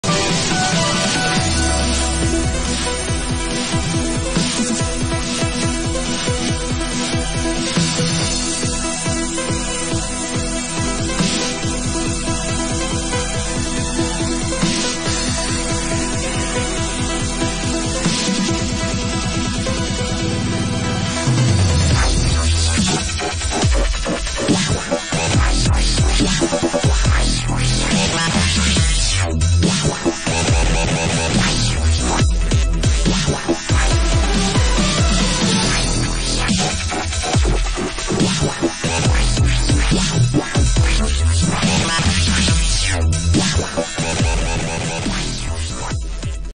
INMOTION V11: Most Advanced & Powerful Electric Unicycle of All Time
Looking for an e unicycle that is both suitable for offroad and on-road commuting?

Choose Inmotion V11 that comes with a bright responsive brake lights, an anti-spin motor kill-switch built in the handles, and extra-wide pedals.